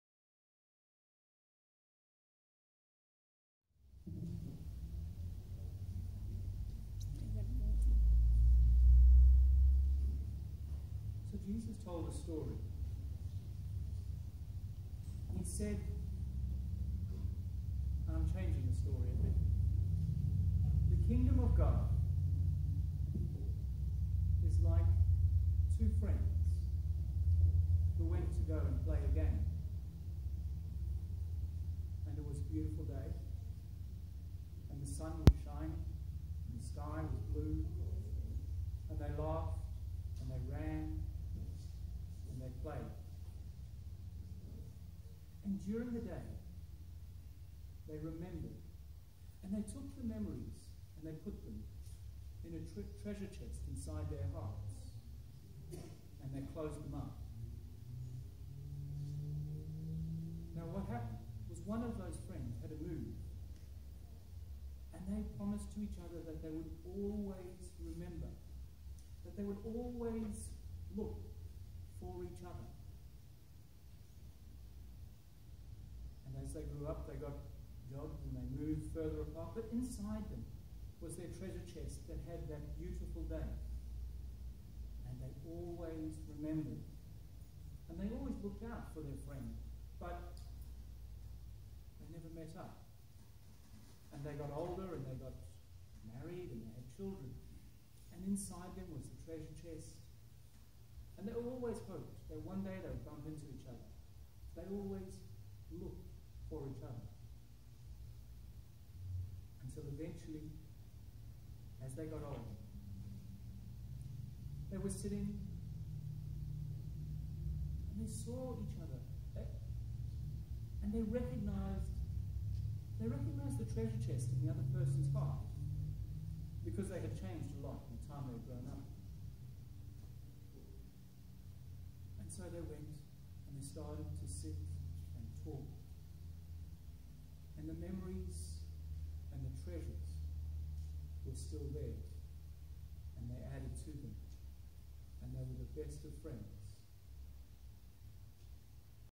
Kids Sermon 30th July 2017
A reflection on Matthew 13.44-46; a children's story about seeking God who seeks us.